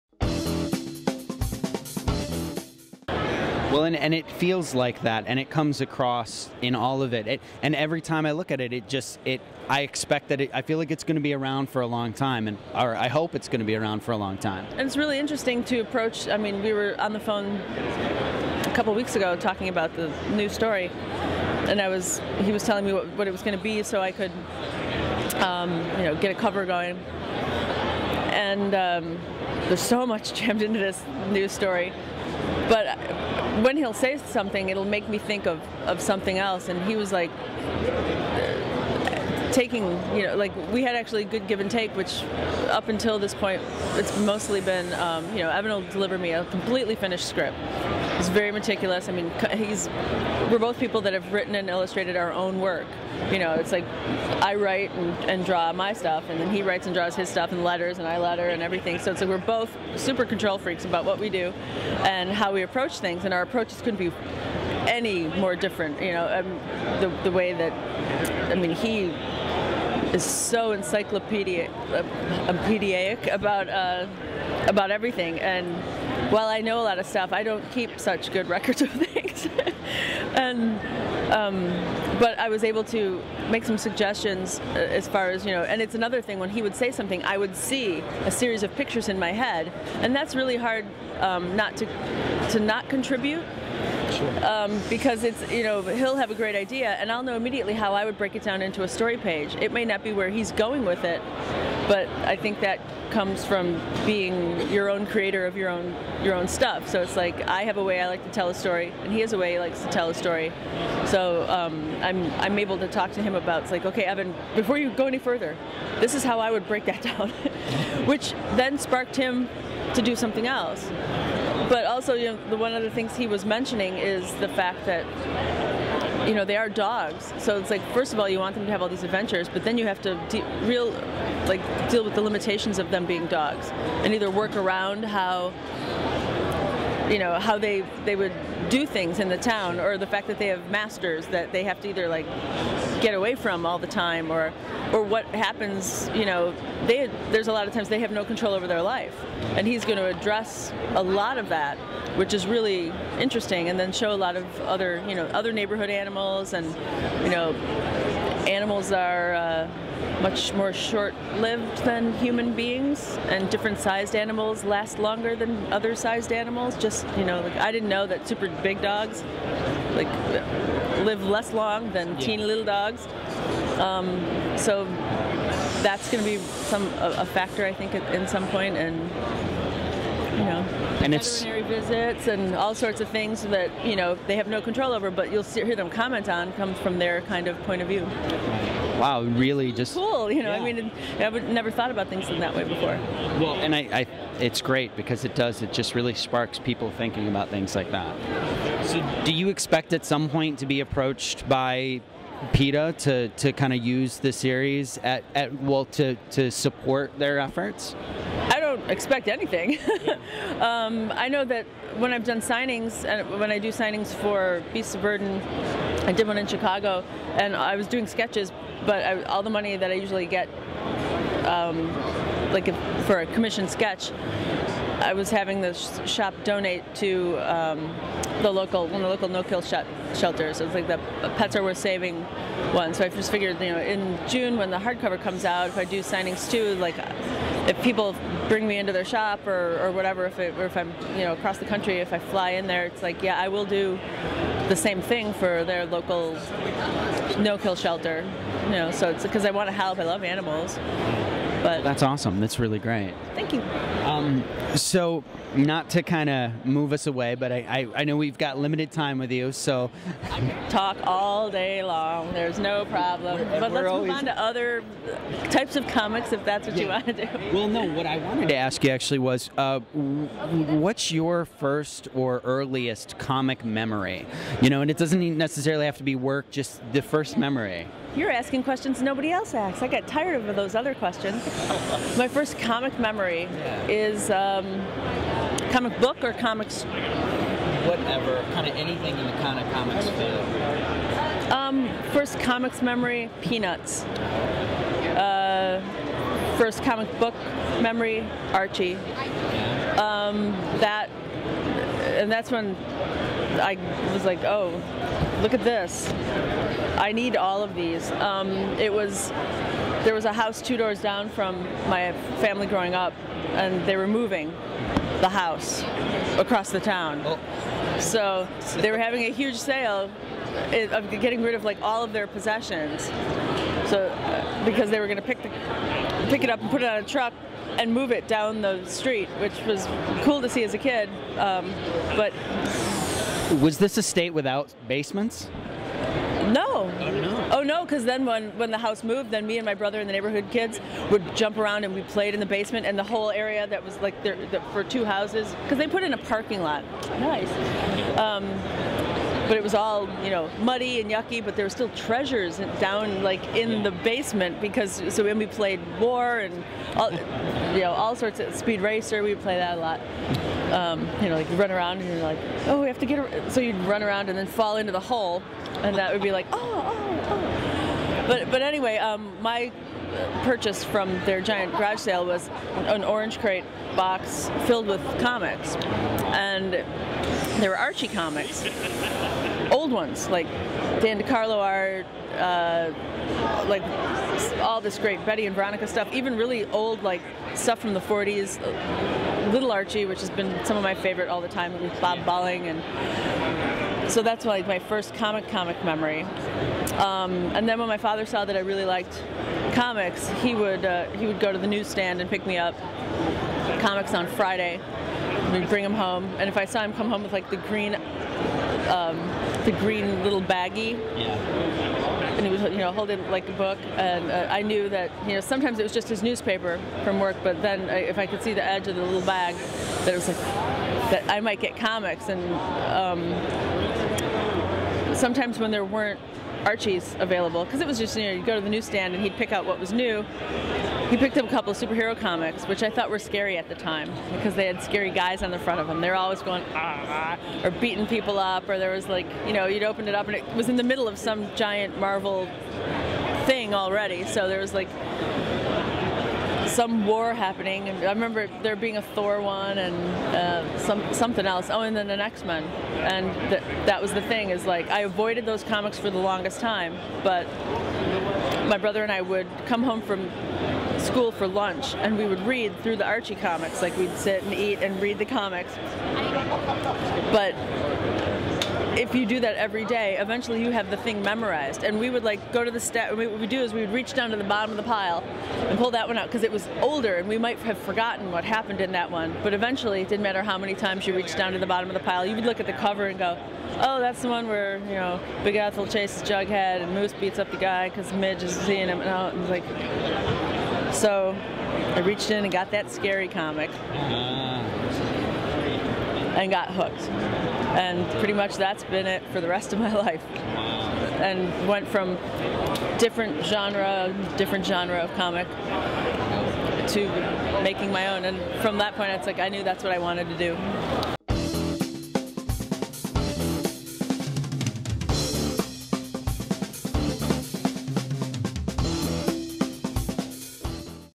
Backroom Comics Podcast - Jill Thompson Interview at ECCC 2010 Part 2
Part 2 of an interview with Jill Thompson, artist on Beasts of Burden, at the 2010 Emerald City ComiCon by the Backroom Comics Podcast.